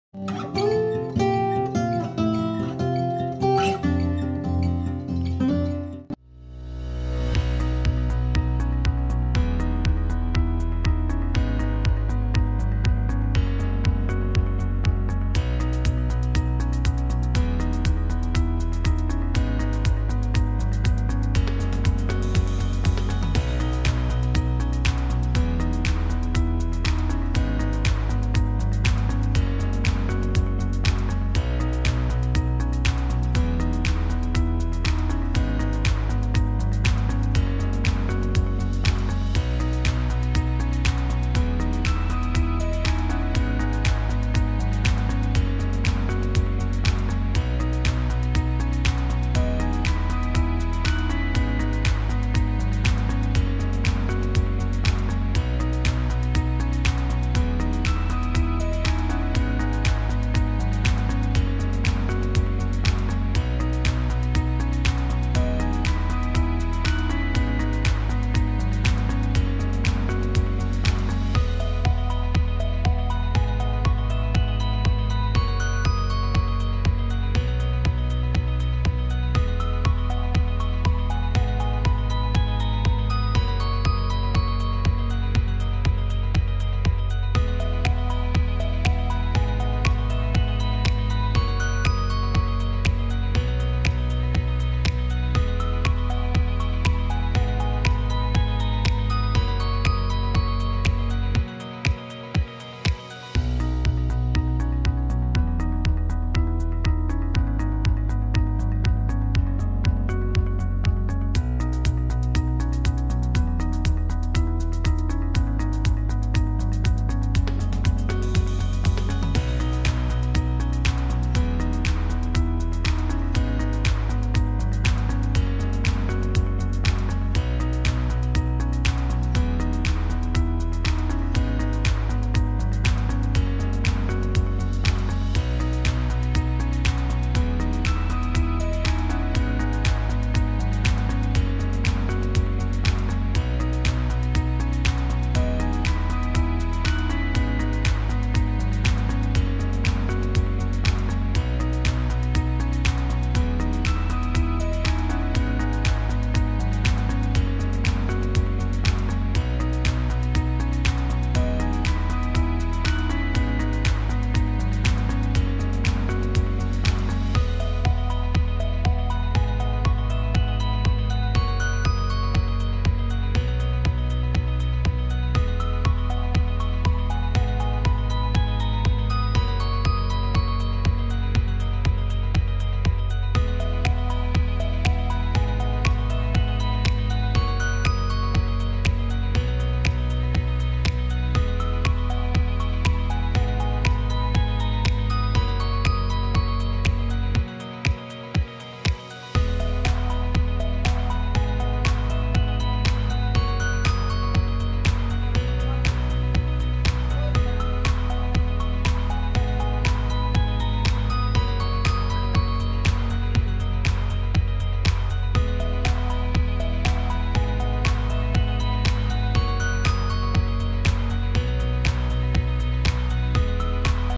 Simple aari work blouse using clip stone for beginners | Livi Fashion
Aari embroidery blouse design using clip stone

---Material Required---

*Aari needle
*Zari thread
*Sugar bead
*Fabric glue
*Wheat bead
*Antic bead
*Clip stone(Triangle shape)